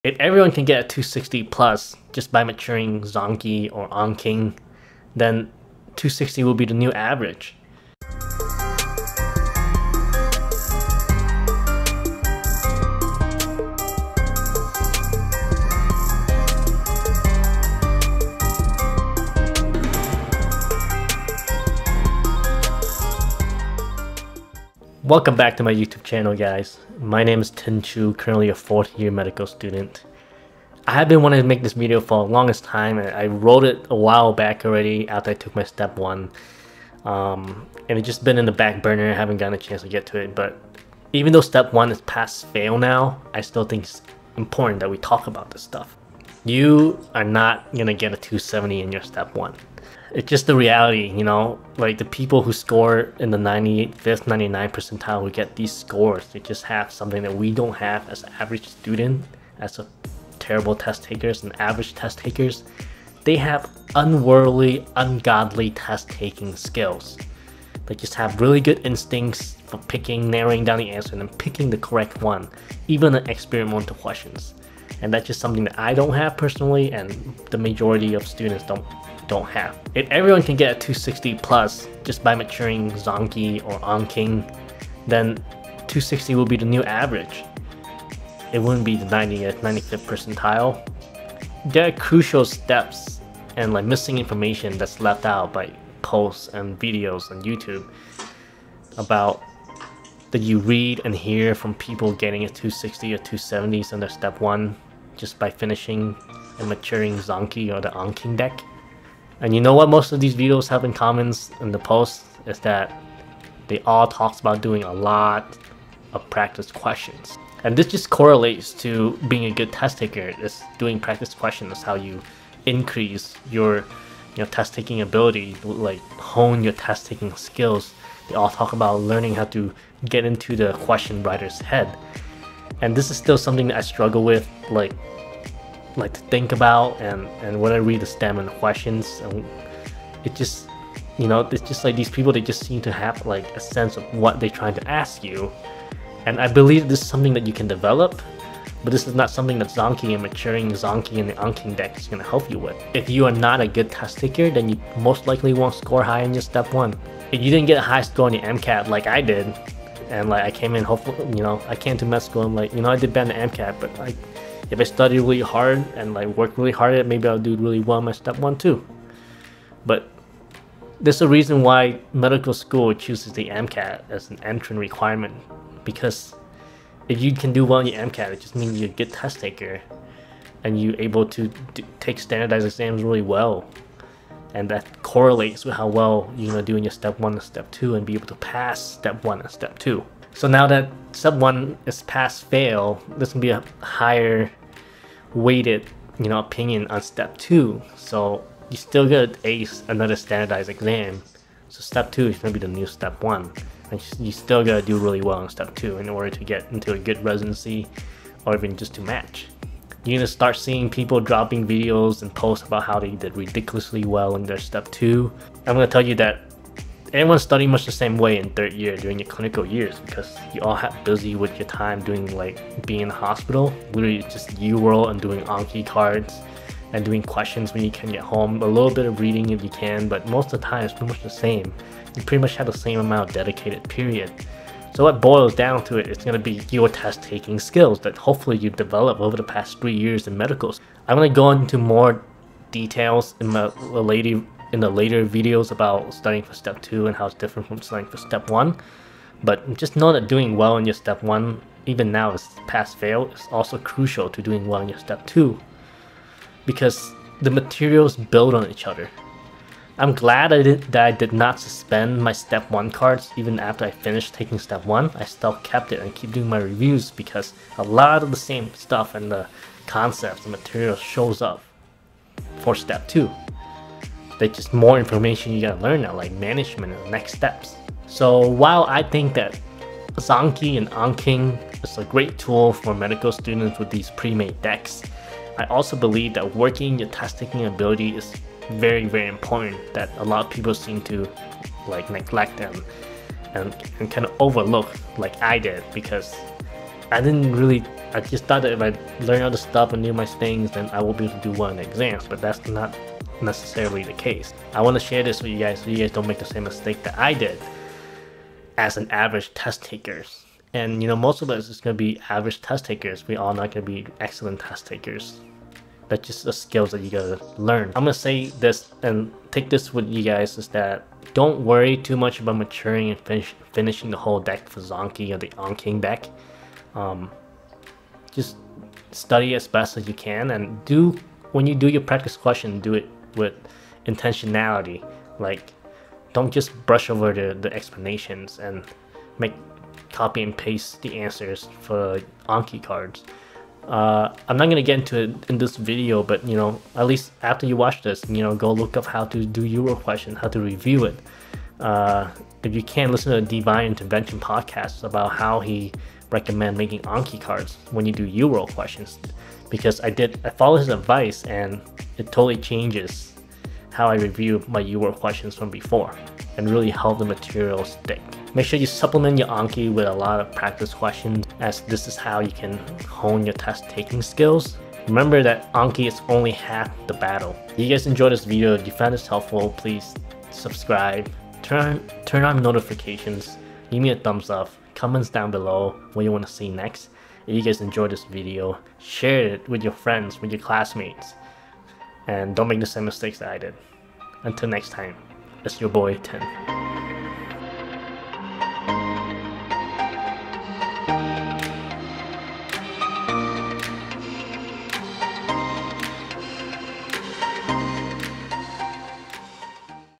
I Matured 90% of Zanki And Didn't Get A 260+ | Med School Tip
-While I think that Zanki and Anking is a great tool for medical students., I also believe that working on your test-taking ability is crucial for doing well on your board exams.
-I want to share this with you guys so you don’t make the same mistake that I did as an average test-taker.
-Don’t worry and stress out too much about maturing and finishing all of Anking or Zanki.
-Supplement your Anki with doing a lot of practice questions as this is how you improve your test-taking skills.

Listen to this to learn how to make Anki cards for Uworld Incorrects